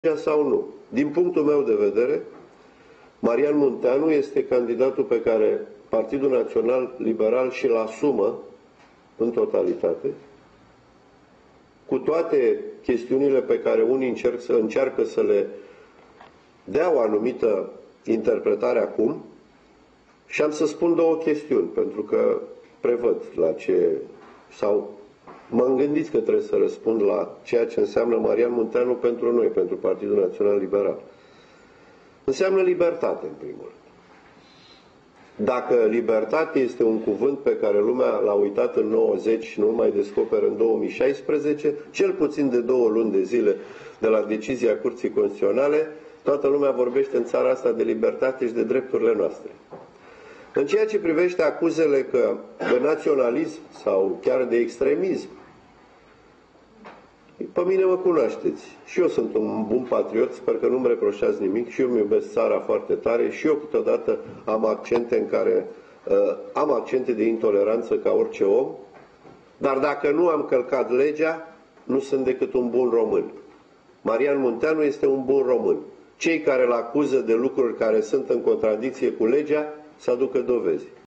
Conferința de presă susținută de Cezar Preda, vicepreședinte PNL, și Dan Cristian Popescu, candid...
Cezar Preda: Marian Munteanu este un bun român. Cei care îl acuză de lucruri care sunt în contradicție cu legea să aducă dovezi.
Titlul evenimentului: Conferința de presă susținută de Cezar Preda, vicepreședinte PNL, și Dan Cristian Popescu, candidat la Primăria Sector 2